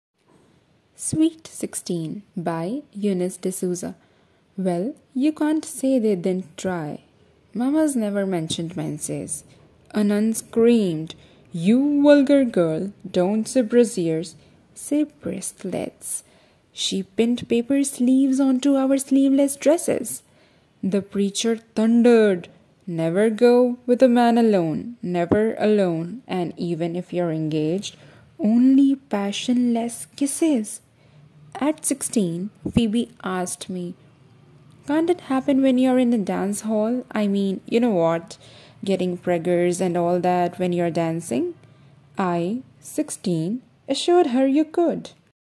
Sweet Sixteen by Eunice De Souza
Sweet Sixteen
by Eunice De Souza

Well, you can’t say
they didn’t try.
Mamas never mentioned menses.
A nun screamed: You vulgar girl
don’t say brassieres
say bracelets.
She pinned paper sleeves
onto our sleeveless dresses.
The preacher thundered:
Never go with a man alone
Never alone
and even if you’re engaged
only passionless kisses.

At sixteen, Phoebe asked me:
Can’t it happen when you’re in a dance hall
I mean, you know what,
getting preggers and all that, when
you’re dancing?
I, sixteen, assured her
you could.